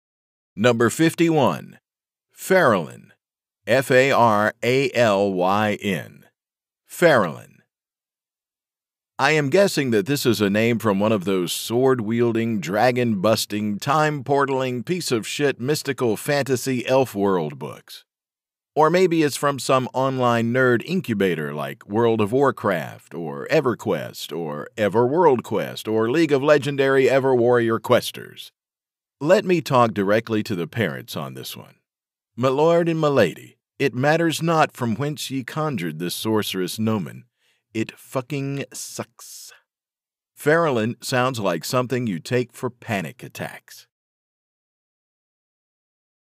Faralyn: 100 Terrible Names For a Baby
Faralyn sounds like something you take for panic attacks. --
Excerpted from "100 Terrible Names For a Baby, Volume 2" on Amazon and Audible